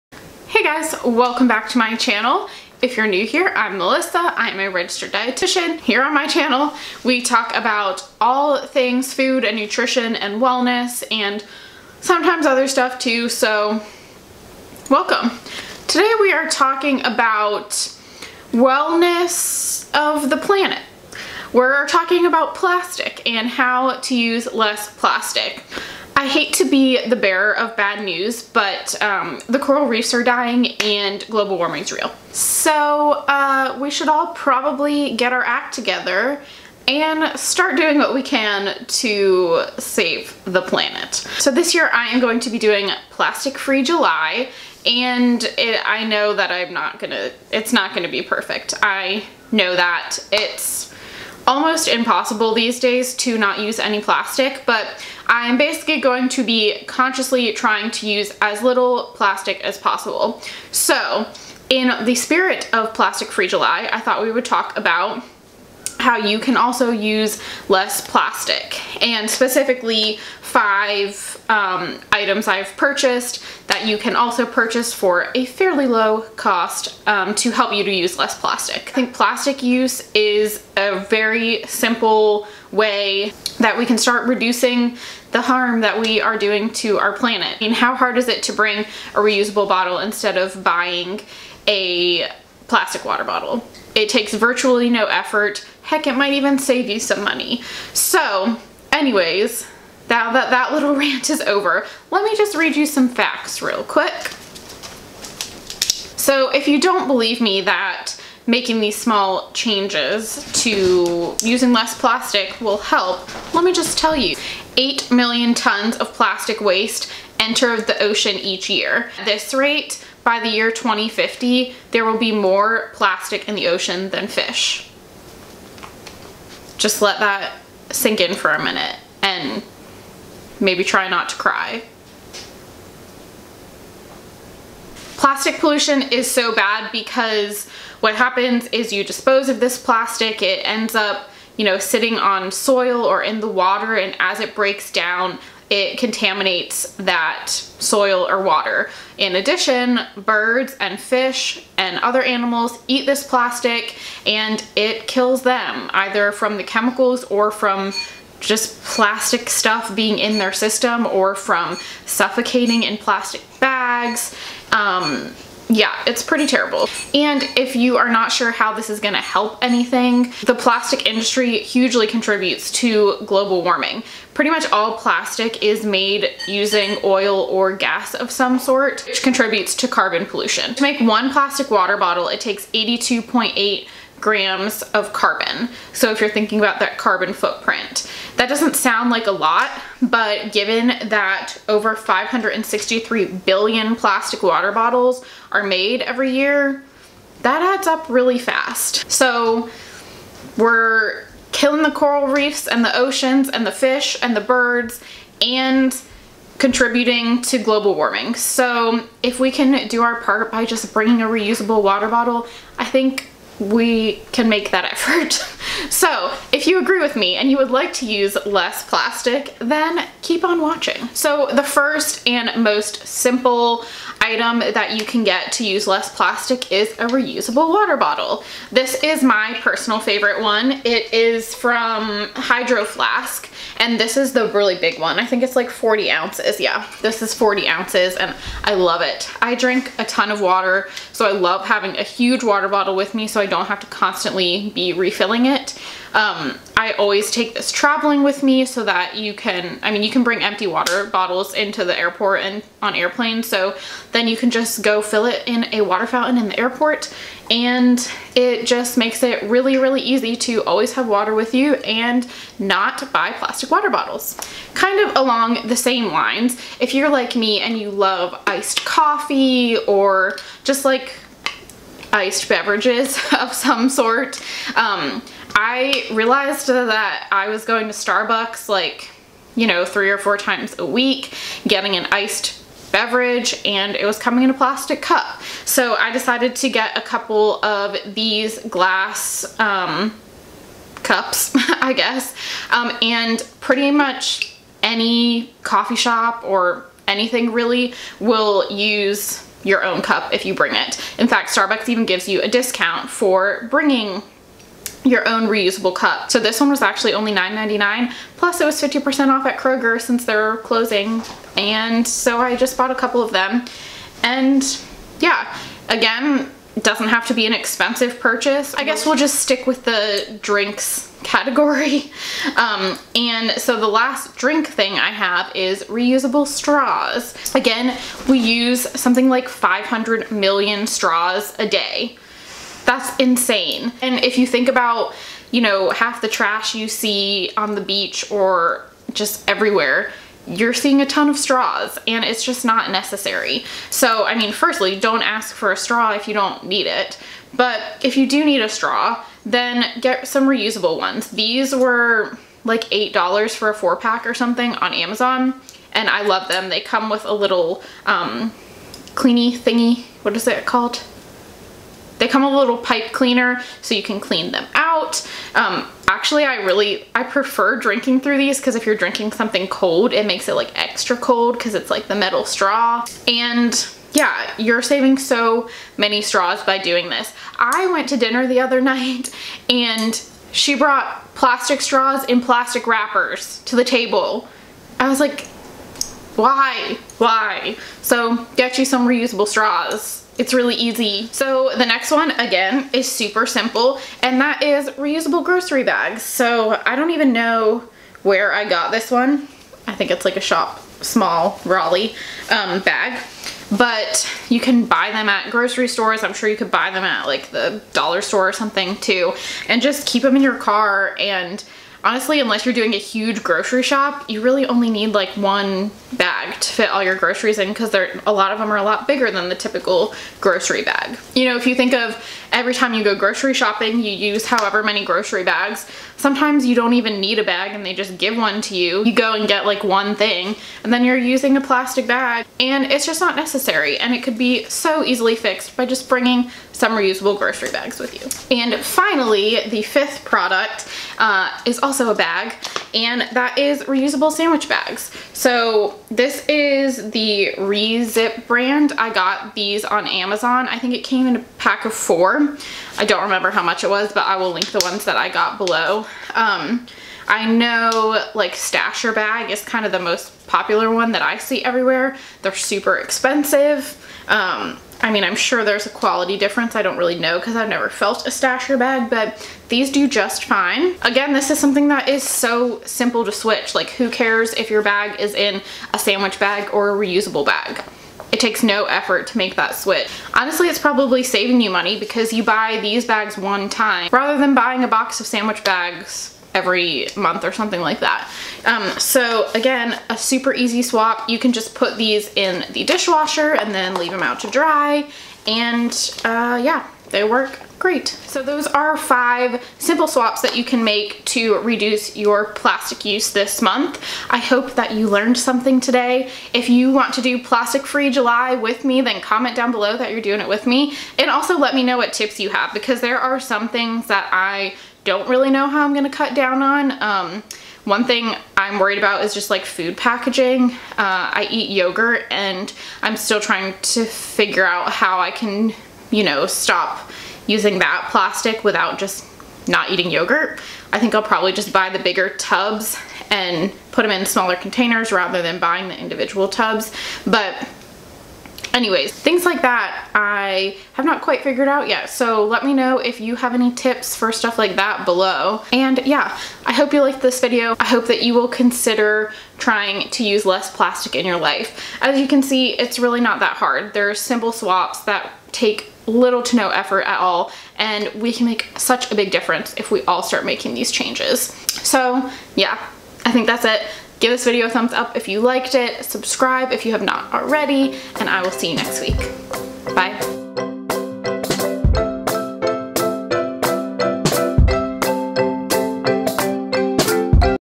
How to Use Less Plastic
Today we're talking all about how to use less plastic!

PRODUCTS MENTIONED
HydroFlask Water Bottle
Iced Coffee Cup
Reusable Straws
Reusable Sandwich Bags